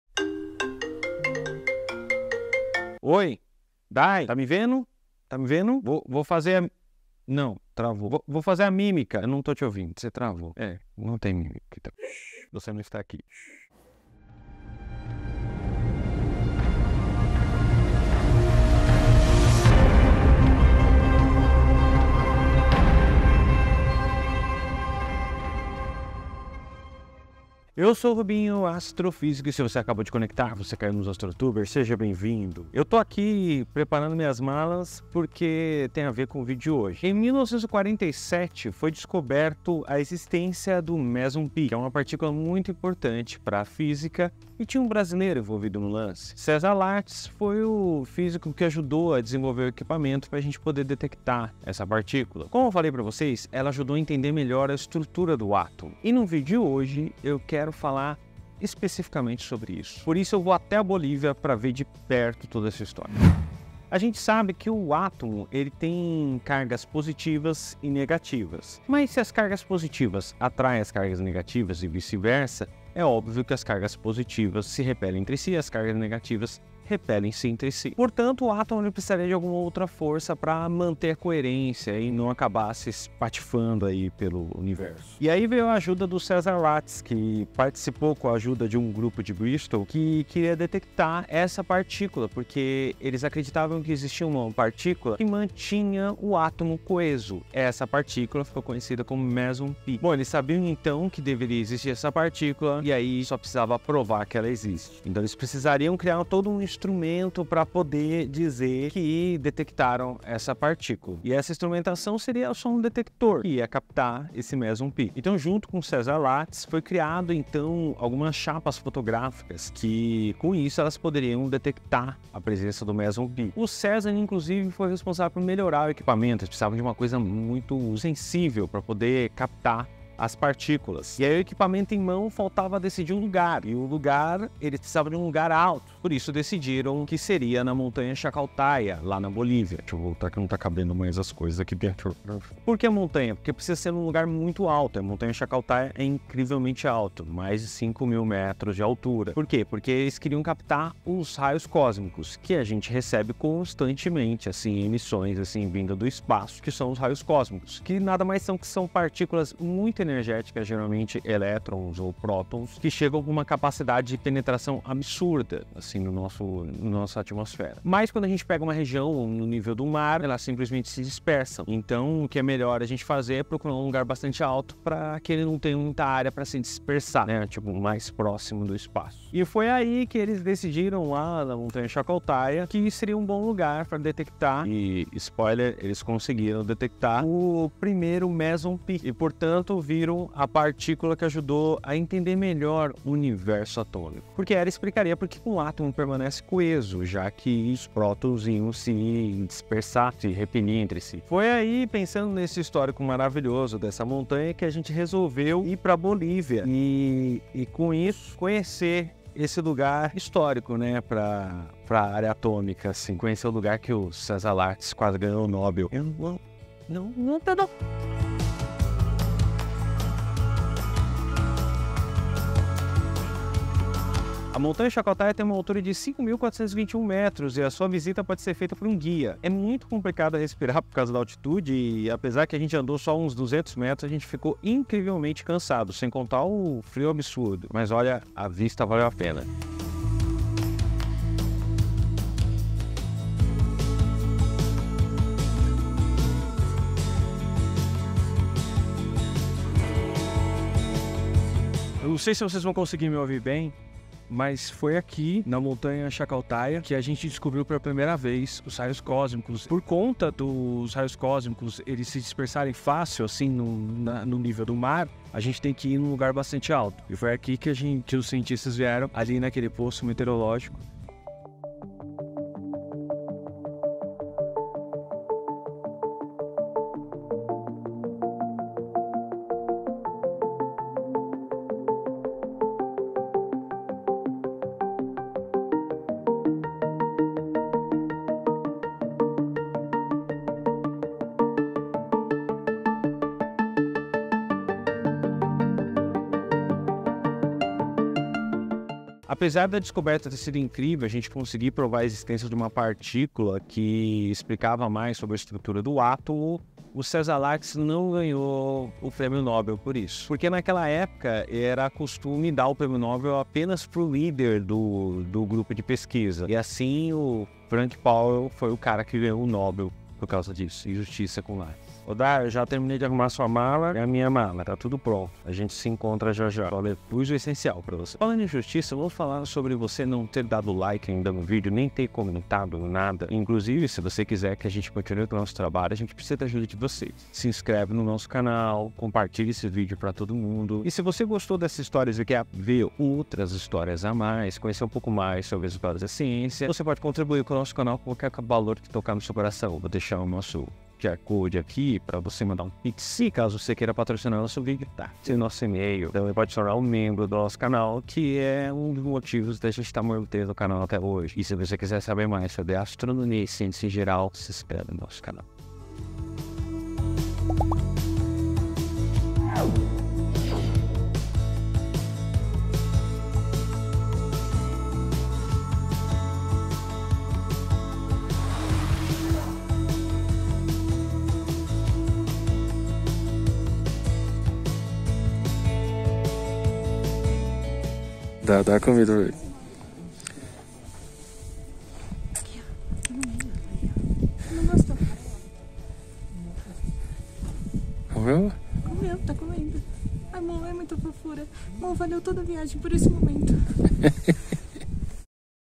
COMO FOI DESCOBERTO O MÉSON-PI
Visitamos a Montanha Chacaltaya, um lugar histórico por ajudar a entender melhor a estrutura do átomo.
Venha com a gente neste pequeno vídeo para entender melhor a história

Ajude o canal fazendo qualquer doação para nosso pix.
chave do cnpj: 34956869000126

Apoiase: /astrotubers